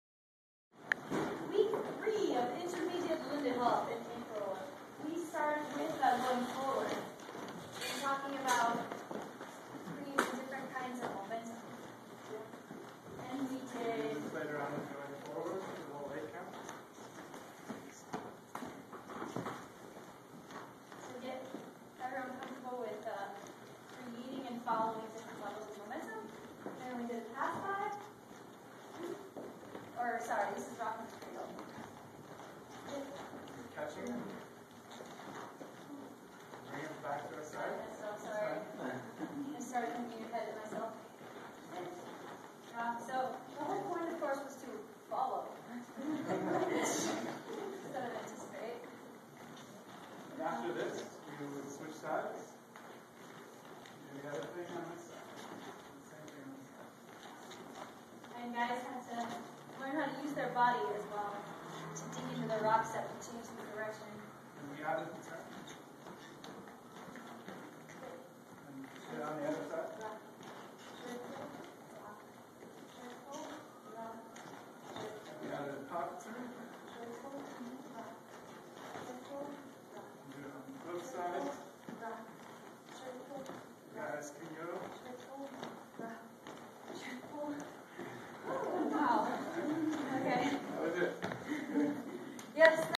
Intermediate Lindy Hop Week 3 April 2011
Taught by Tiffiny Wine and Ceth Stifel at the Mercury Cafe in Denver, Colorado.